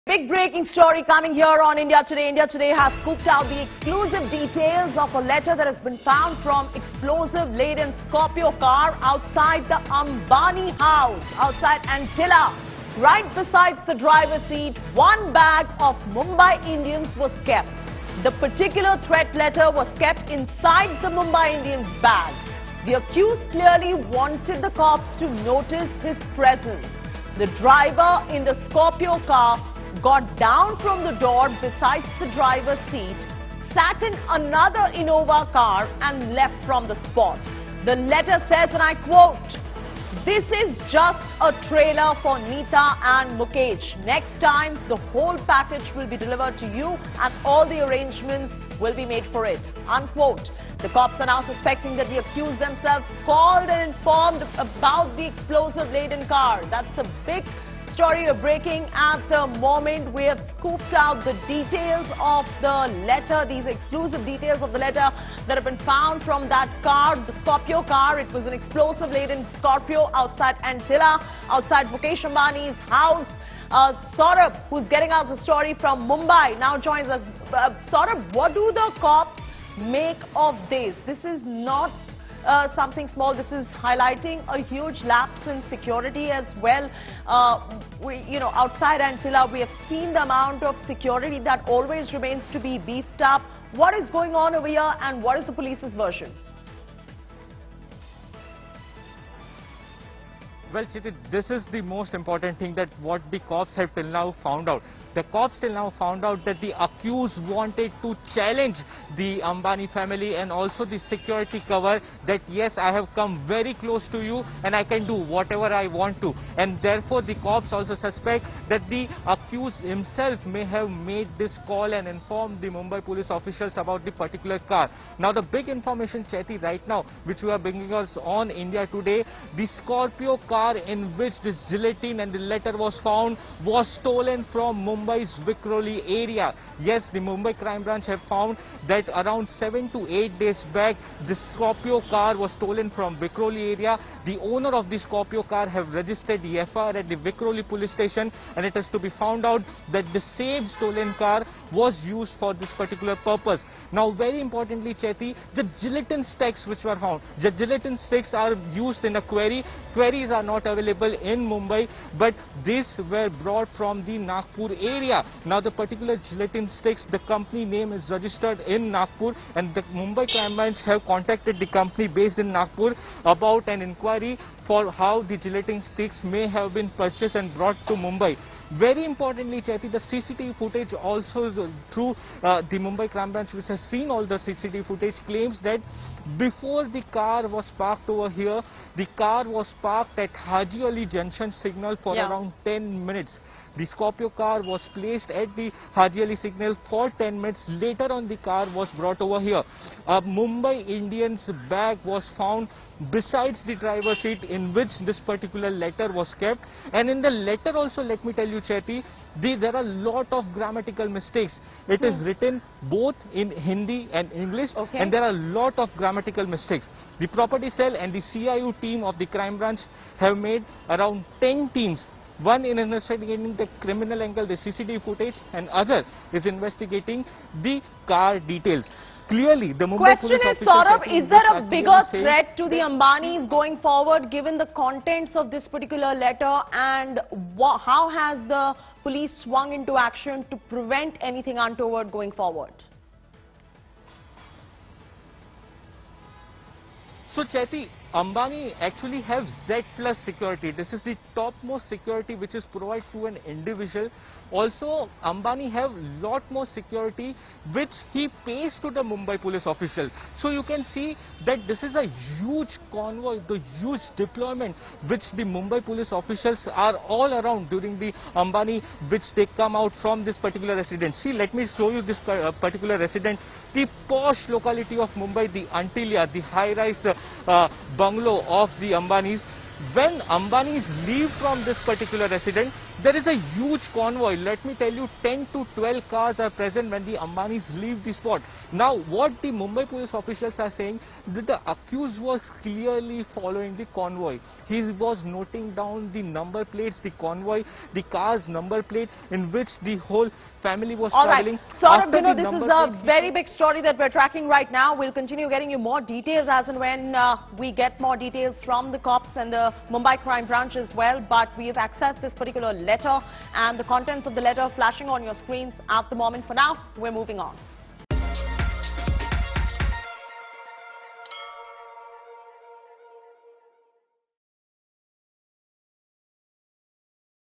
Threat letter recovered in bag near Mukesh Ambani's house 'Antilia'
Mumbai Police on Thursday recovered a threat letter, stating ‘Yeh sirf trailer hai (This is only a trailer)’ and 20 explosive gelatin sticks from the abandoned SUV near Mukesh Ambani's residence 'Antilia' in Mumbai. The content of the letter, kept next to the driver's seat in a 'Mumbai Indians' bag, was made public on Friday. Addressing the Ambani family, the threat letter further goes on to say that arrangements had been made and next time 'the material (apparently the explosive gelatin sticks) will reach the family in a developed state.’ Watch the video for more.